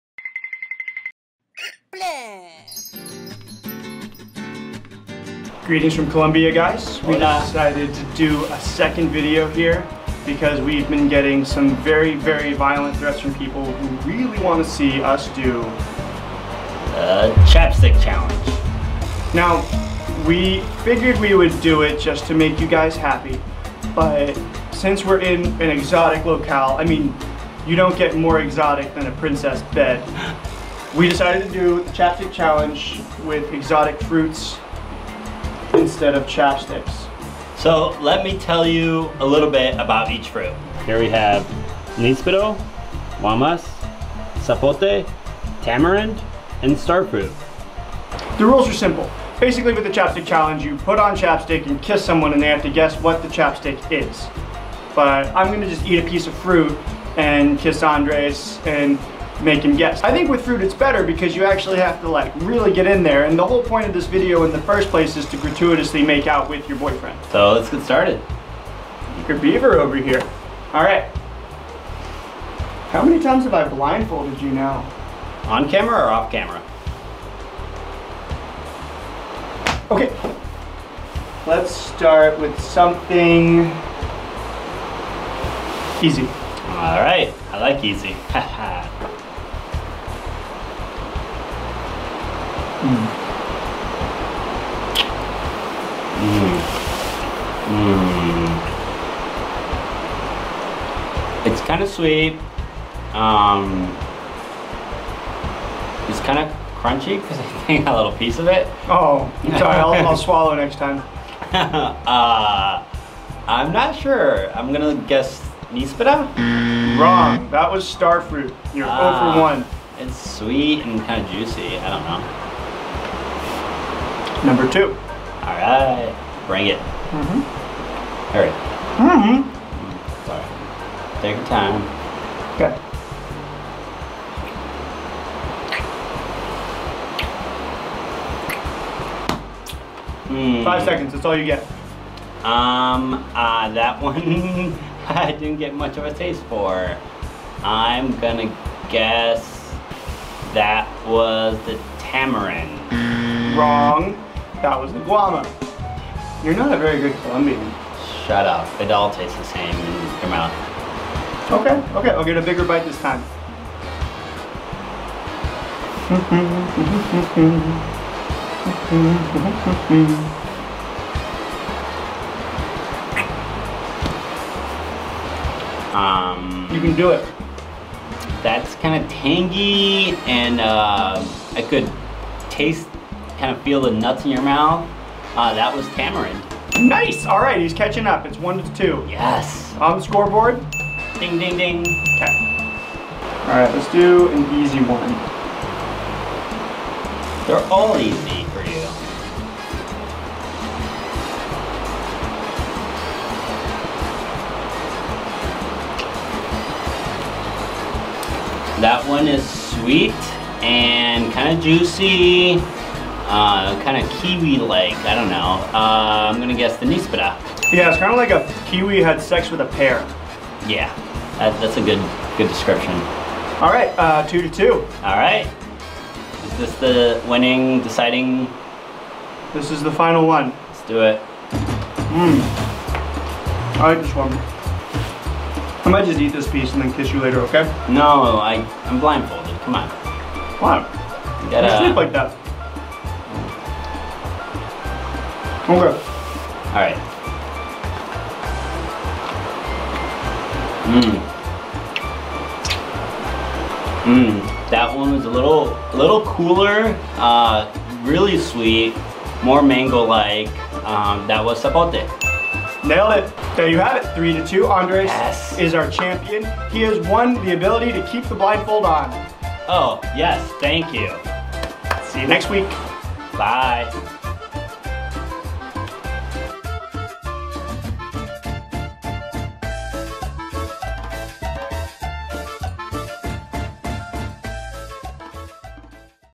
Chapstick Challenge (with Colombian fruit)
We're putting a Colombian twist on the Chapstick Challenge.